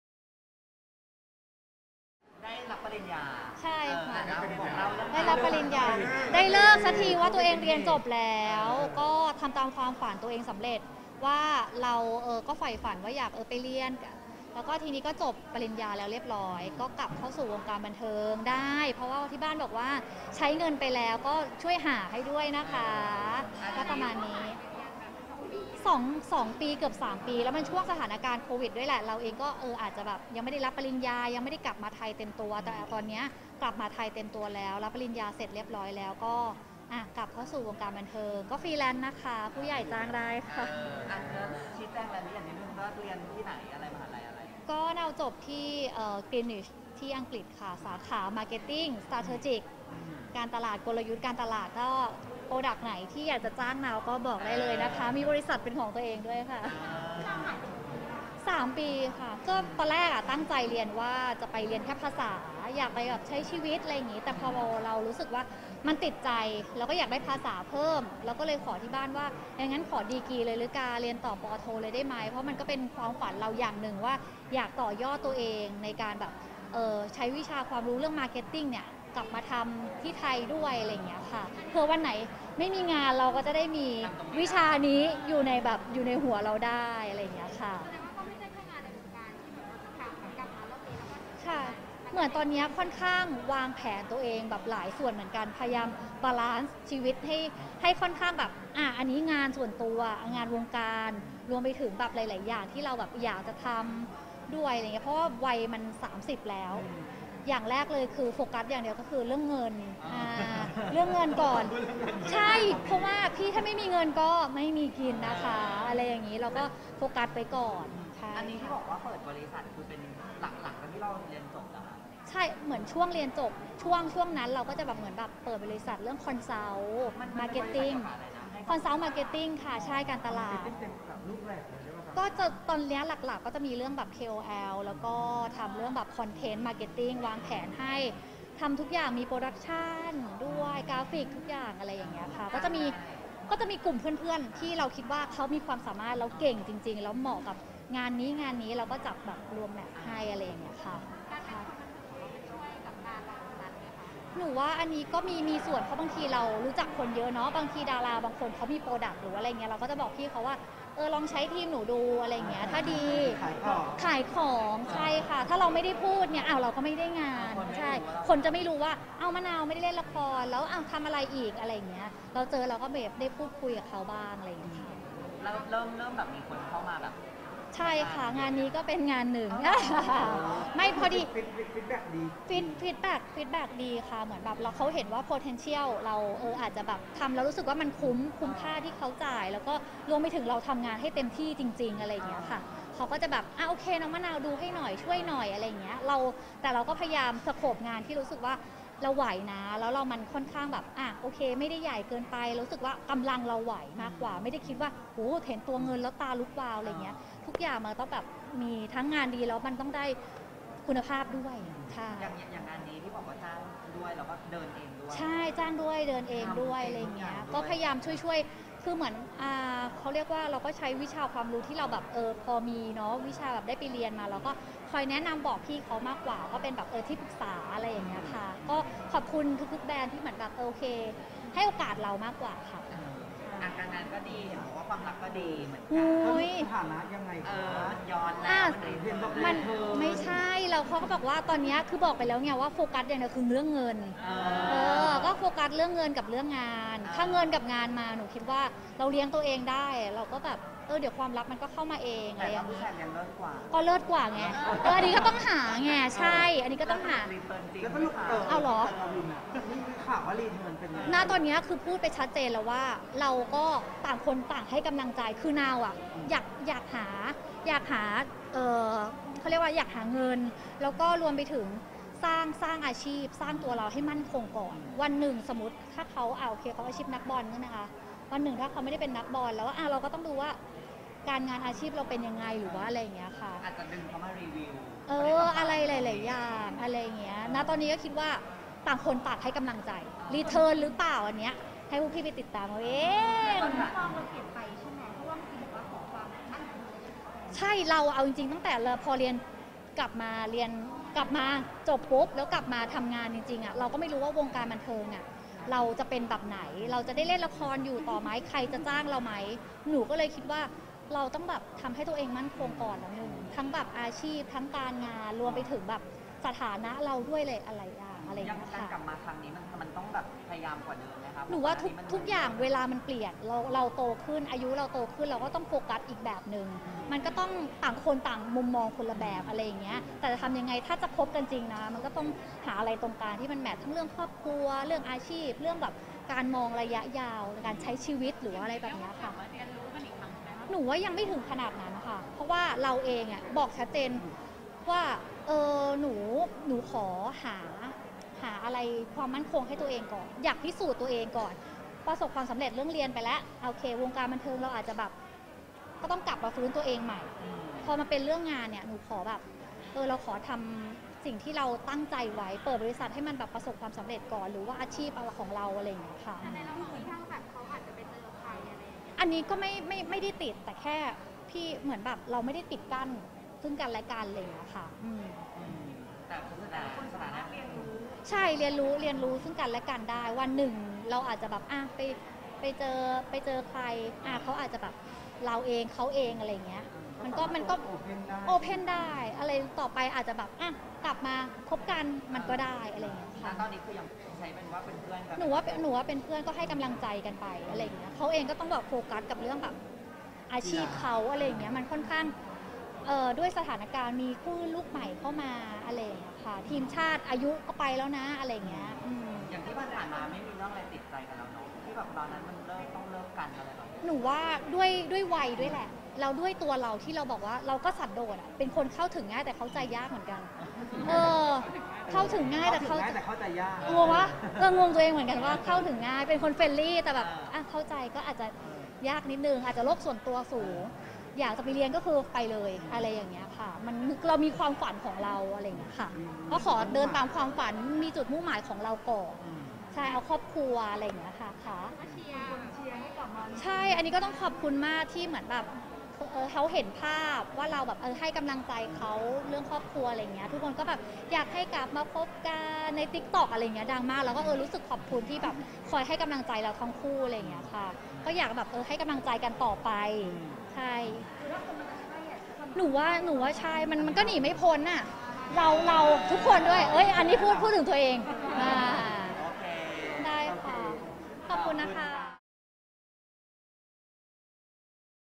มะนาว ศรศิลป์ ดีใจจบป.โท การตลาด University of Greenwich ที่อังกฤษ
มะนาว ศรศิลป์ ดีใจจบป.โท การตลาด University of Greenwich ที่อังกฤษ บอกตอนนี้กลับมาลุยงานวงการแล้ว ในฐานะนักแสดงอิสระ เผยเปิดบริษัทกับเพื่อน อยากใช้ความรู้ที่มีสร้างตัวอีกช่องทางหนึ่ง โต้คืนดีกวินท์ แจงแค่คอยให้กำลังใจ ต่างฝ่ายต่างโฟกัสงานที่ทำ สถานะตอนนี้คือเพื่อน ส่วนอนาคตตอบไม่ได้ ขอบคุณแฟนคลับเชียร์ให้คืนดี